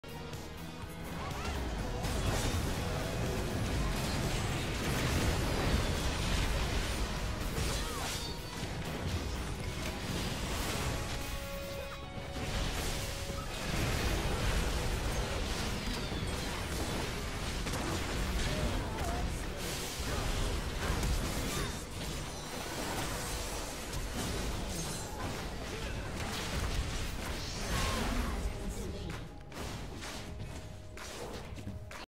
Huge Teamfight
Well played by all on my team :) turned the game -- Watch live